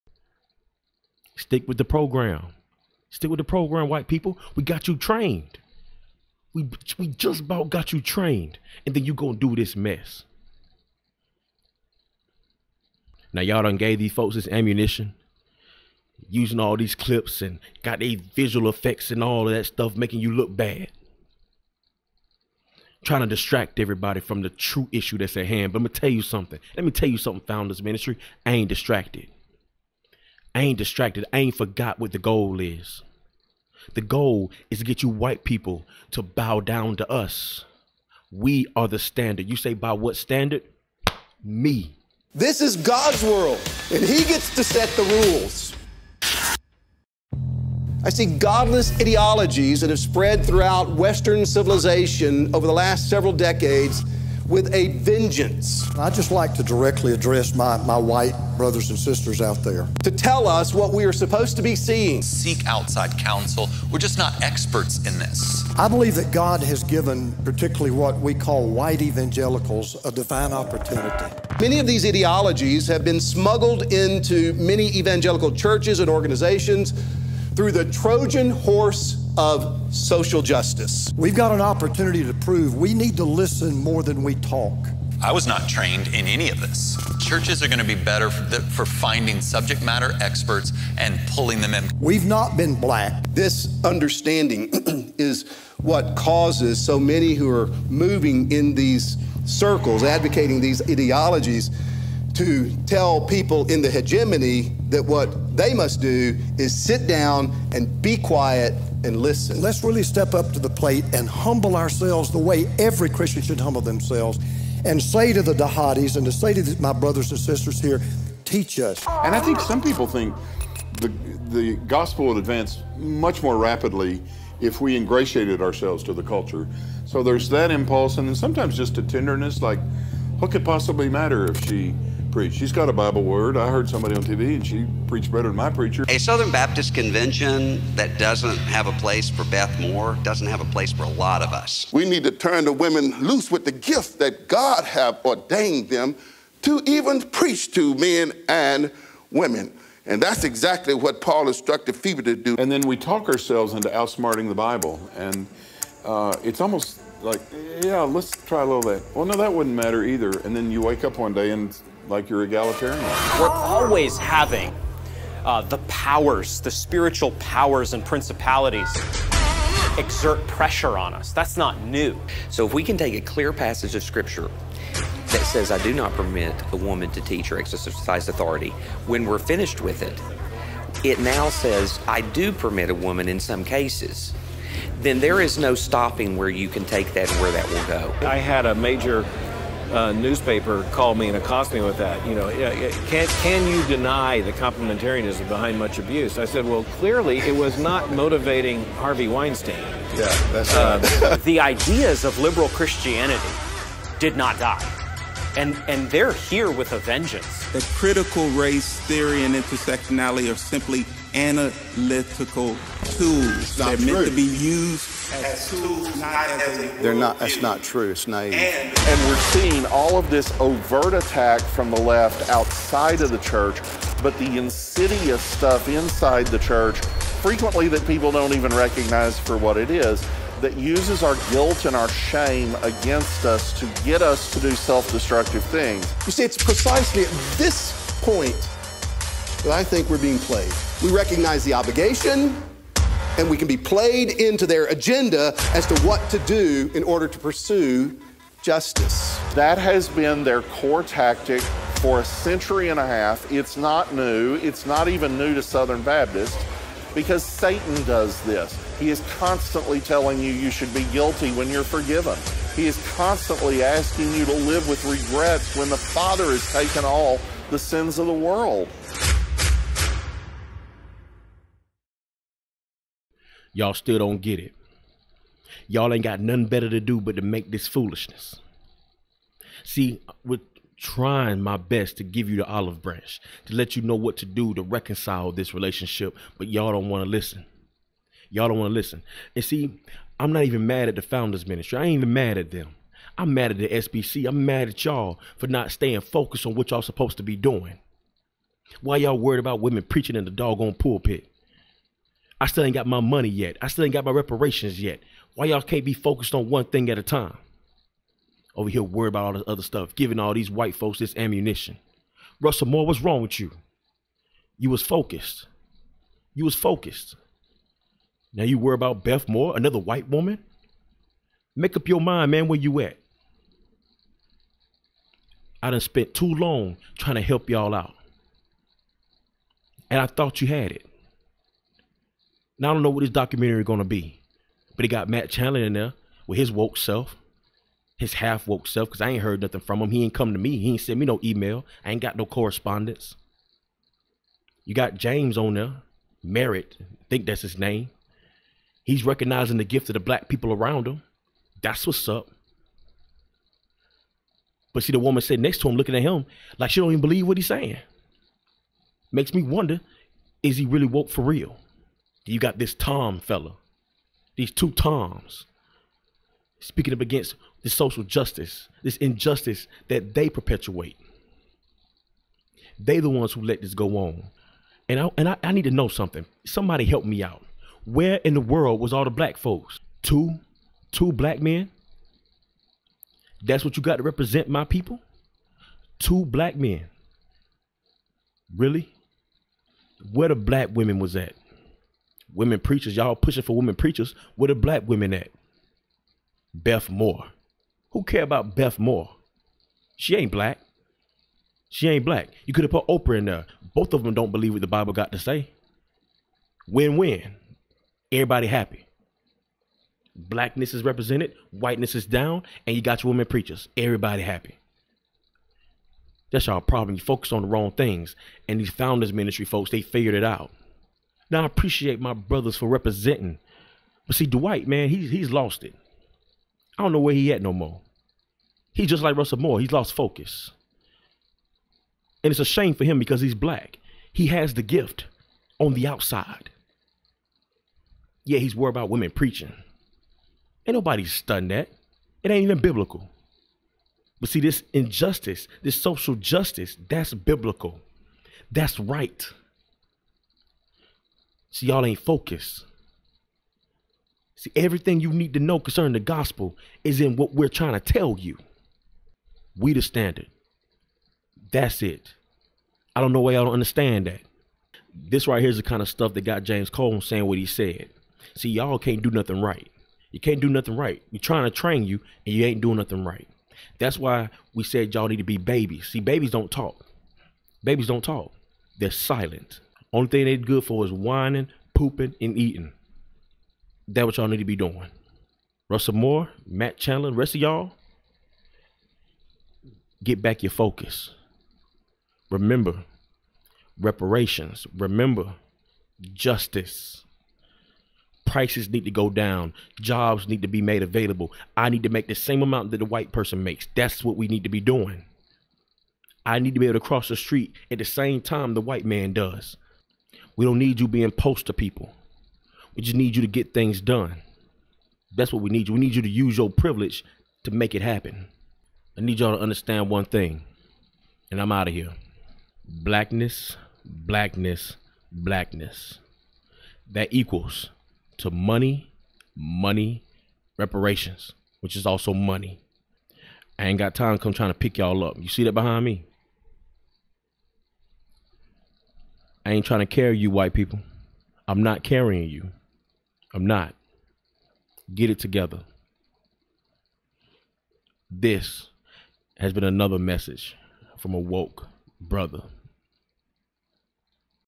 A Woke Brotha Responds To Founders Ministries' "By What Standard?" Trailer
The Founders Ministries released a trailer for their cinedoc called "By What Standard?". A Woke Brotha has a thing or two to say to the SBC because of this release.

Listen to other podcasts on the Christian Podcast Community: ChristianPodcastCommunity.org